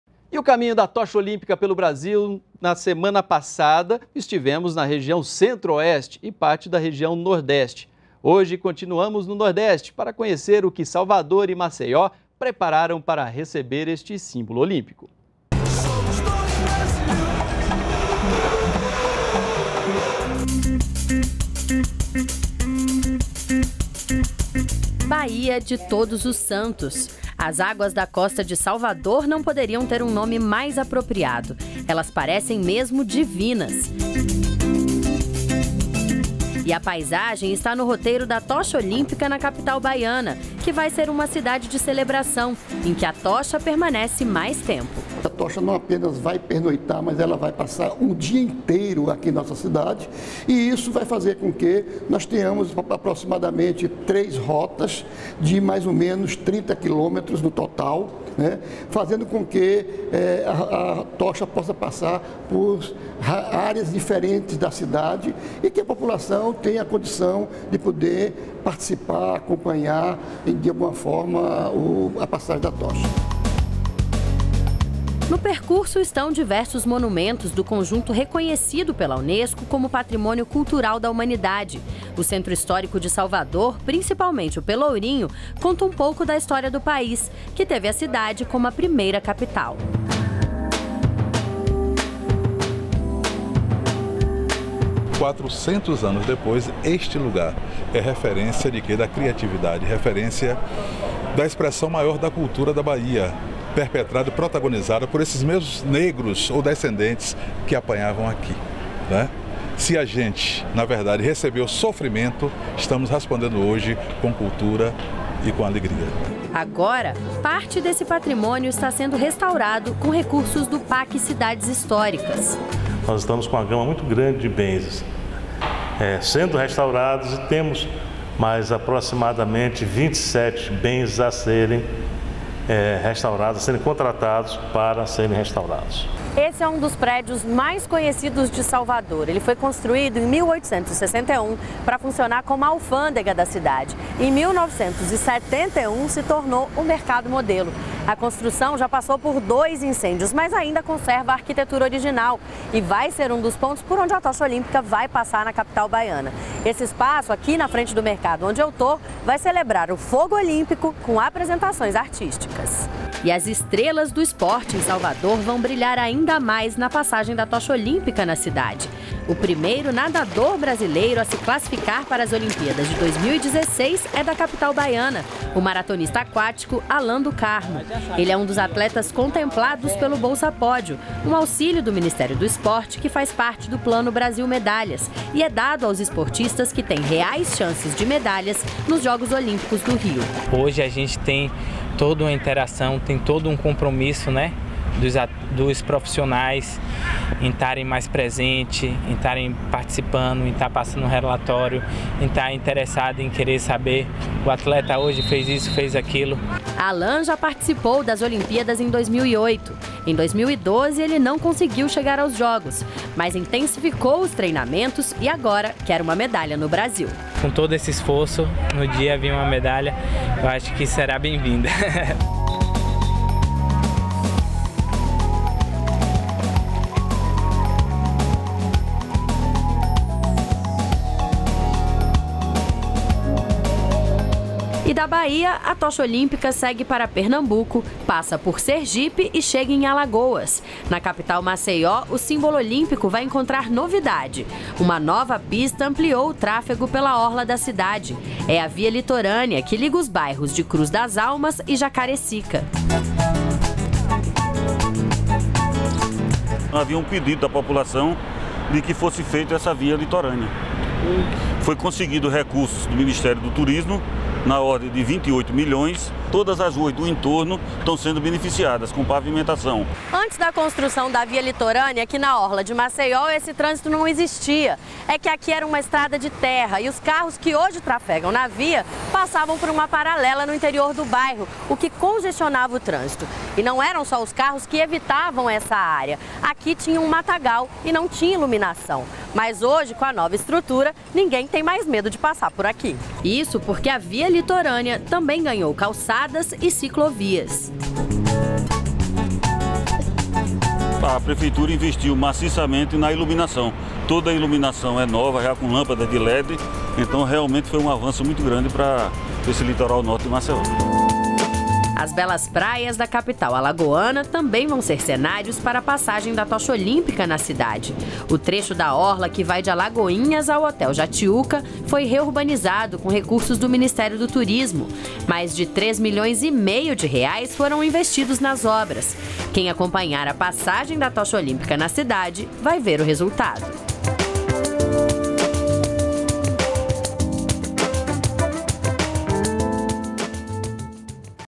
Salvador e Maceió montam programação especial para receber a tocha olímpica
NBR NOTÍCIAS - 25.04.16: A série especial do NBR Notícias continua fazendo o caminho da tocha olímpica pela região Nordeste. Agora, é a vez de Salvador (BA) e Maceió (AL) apresentarem o que prepararam para receber a chama.